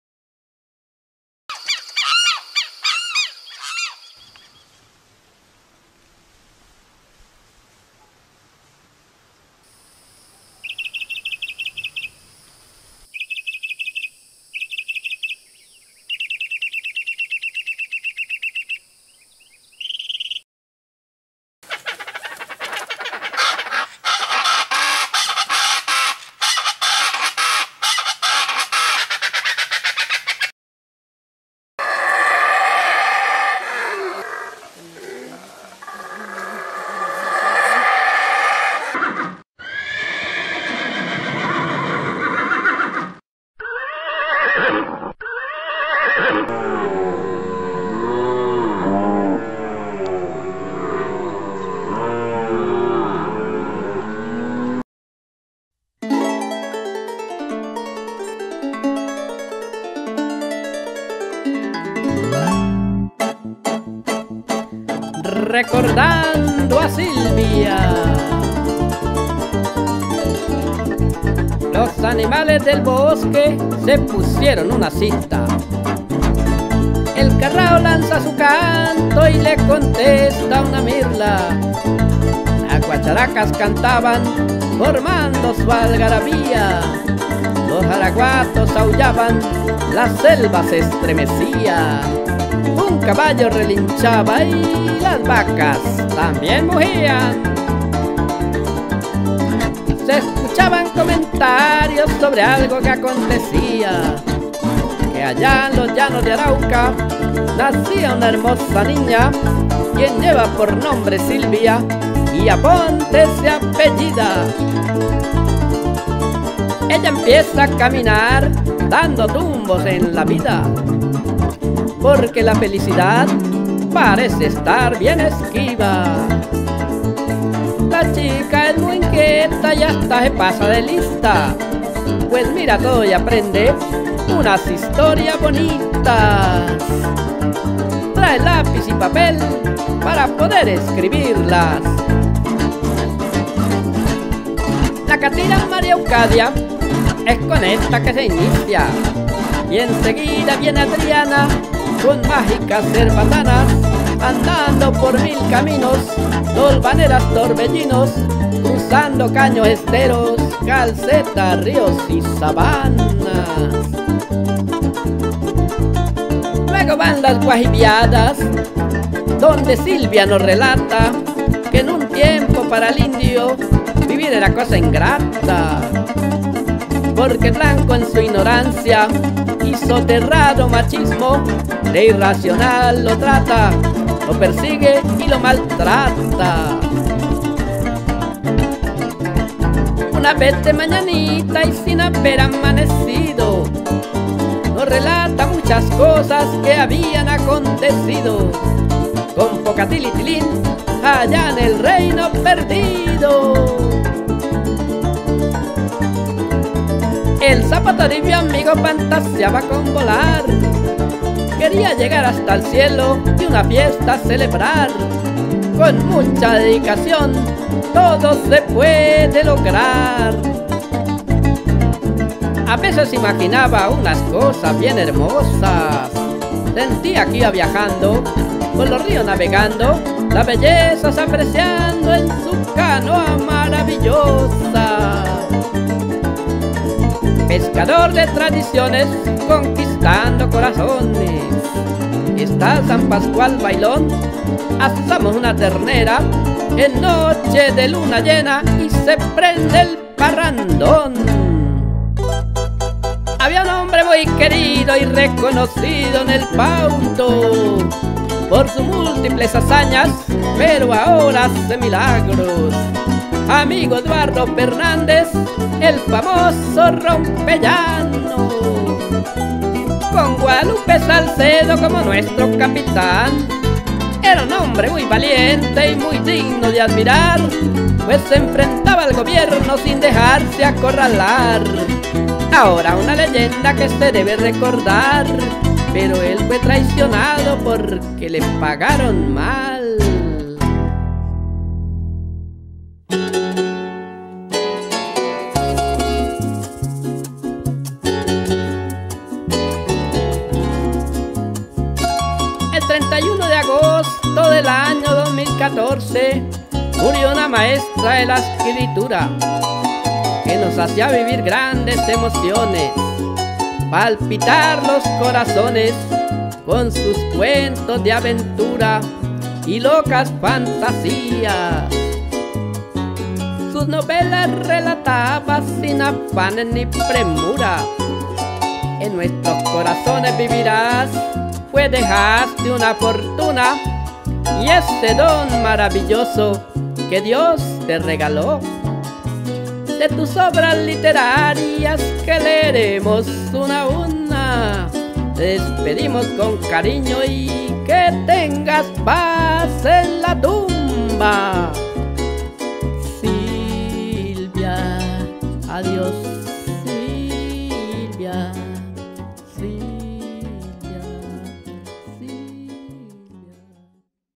Homenaje a: Silvia Aponte de Torrez.
Con este tema, trato de hacer una breve reseña, sobre: la vida y obra de mi colega: Silvia Aponte de Torres. Escritora colombiana, nacida en: Puerto Rondón, Arauca.